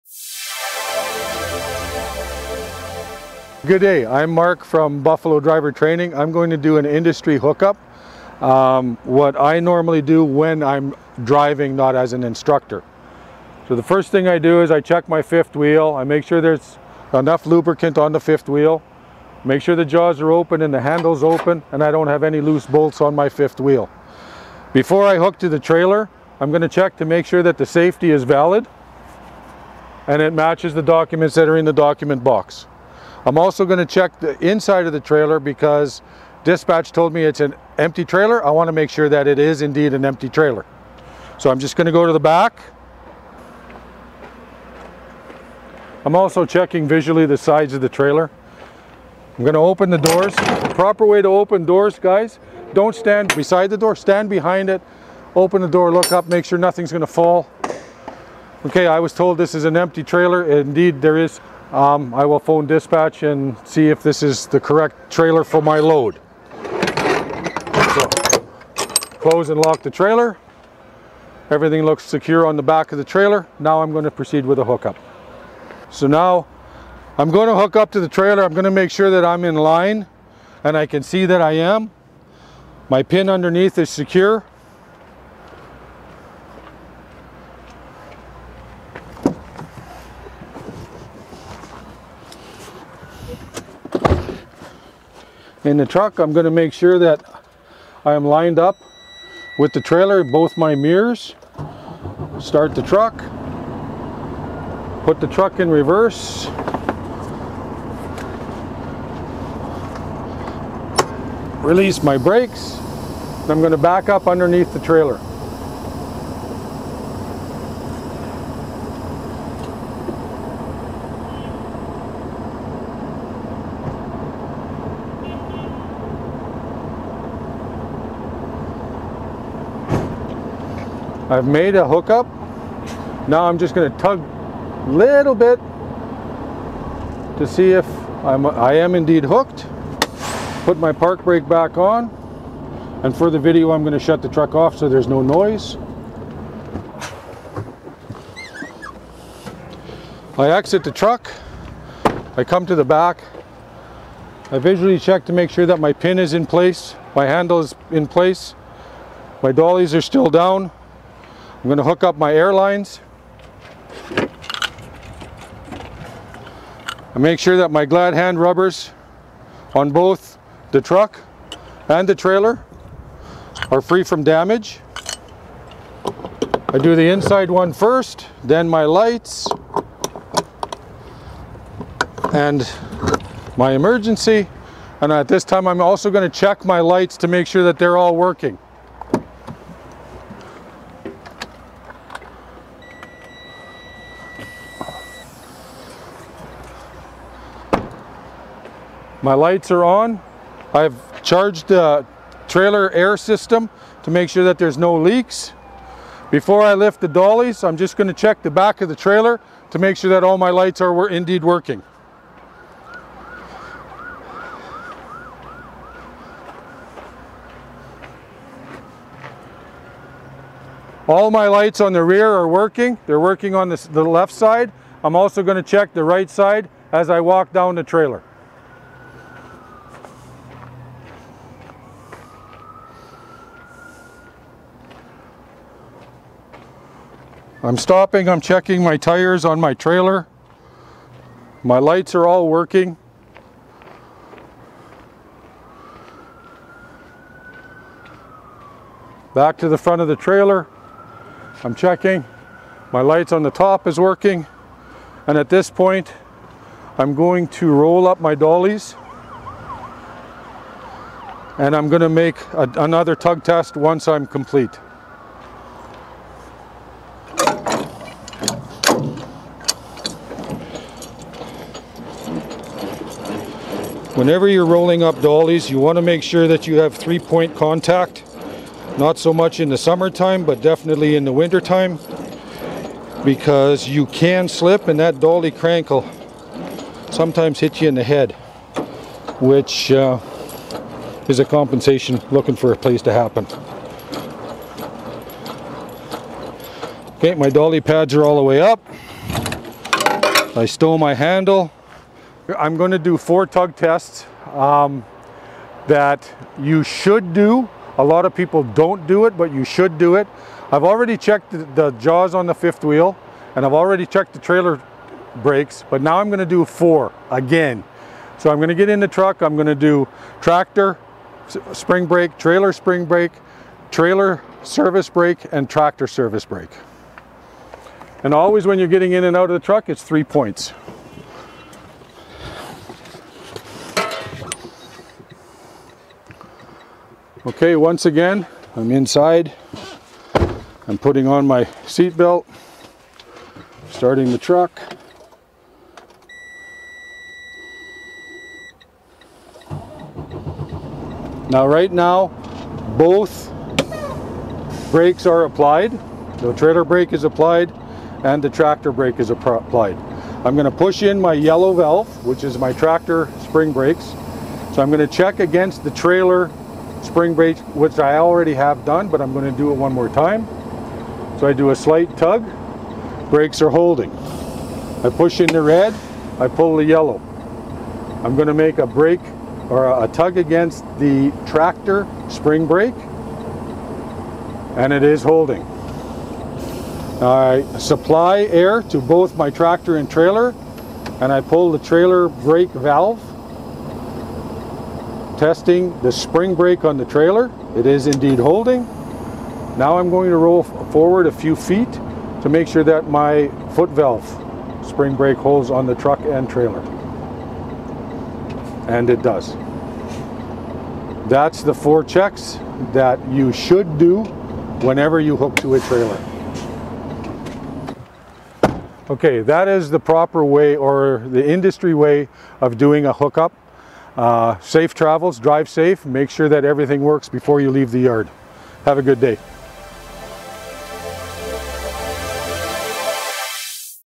Industry Standard Coupling as It's Done in Real Life
There are a lot of YouTube videos about coupling trucks and trailers, but none of them mention how important it is to check the trailer before hooking up. Is it in working condition and clean inside, empty or loaded, depending on the situation? Does it have a safety sticker that is not expired and matches the papers in the tube? Is the license plate the same as in the documents? So, be professional and drive safely!